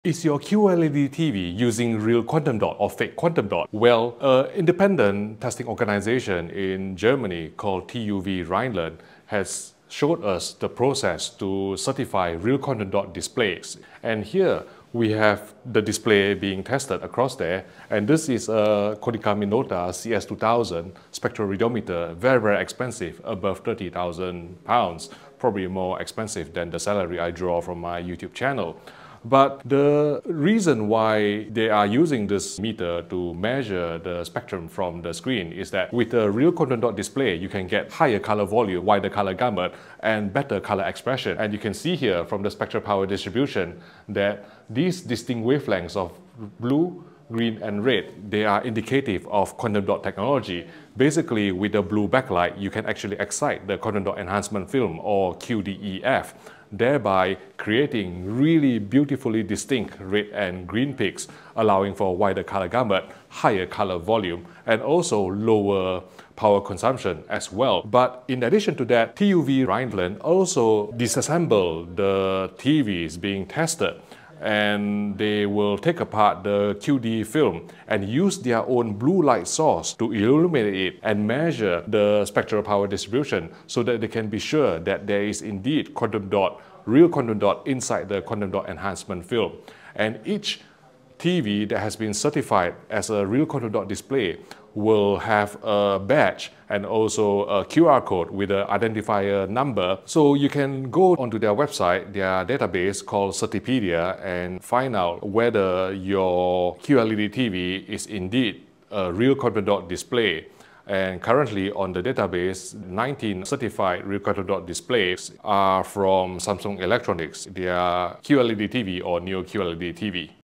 Is Your QLED TV Real or Fake? 👀
How to tell if your QLED TV is using real or fake quantum dot technology, based on testing by TÜV Rheinland, an independent non-profit organisation headquartered in Germany.